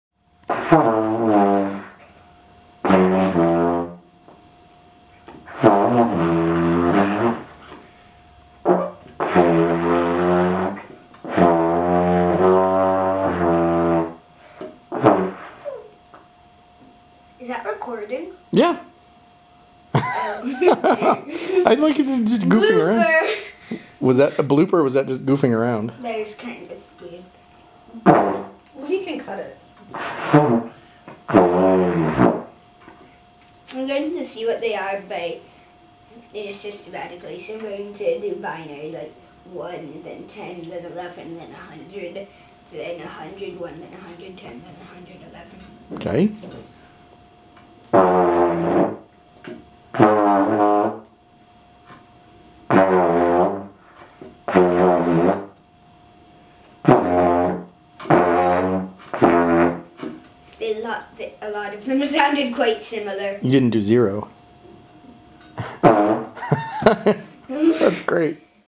Baritone horn binary
In case you're wondering the binary governs which valves are being pressed -- 001, 010, 011, 100, 101, 110, 111. I reminded him to do 000, too.